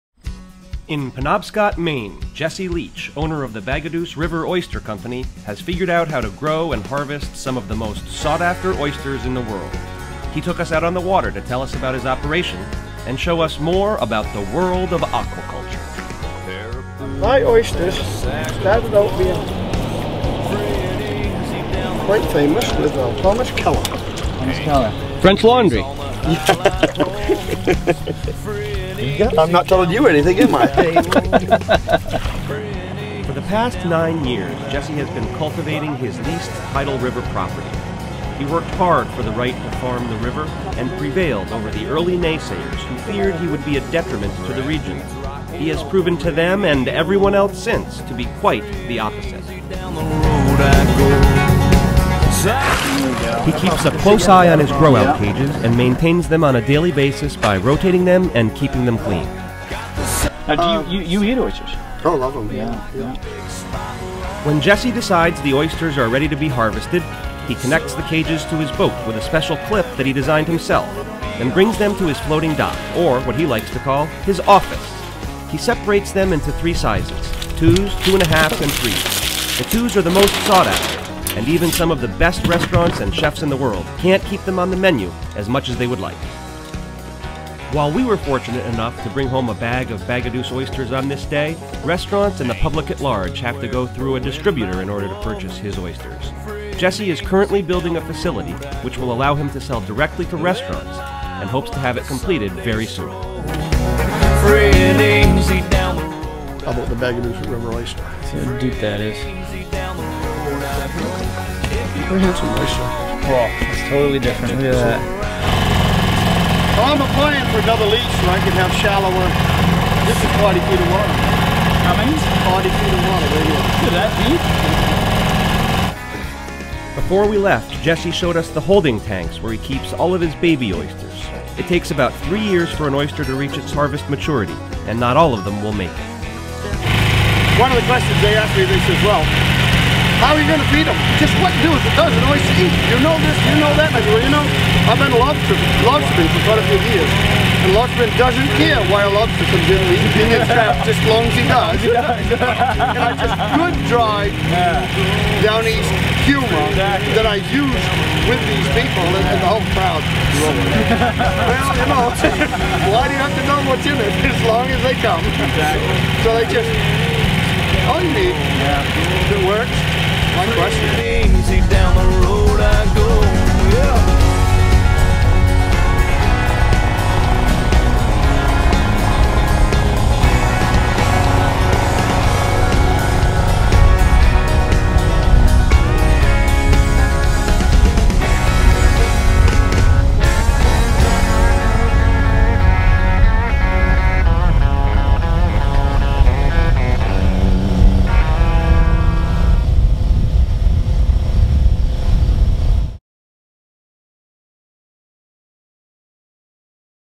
Bagaduce HD.mov
The Bagaduce River Oyster Co. in Penobscot, Maine produces some of the world's best and most sought after oysters. We spent a day with the owner, Jesse Leach, this summer to find out more about his operation and to learn about the growing field of aquaculture.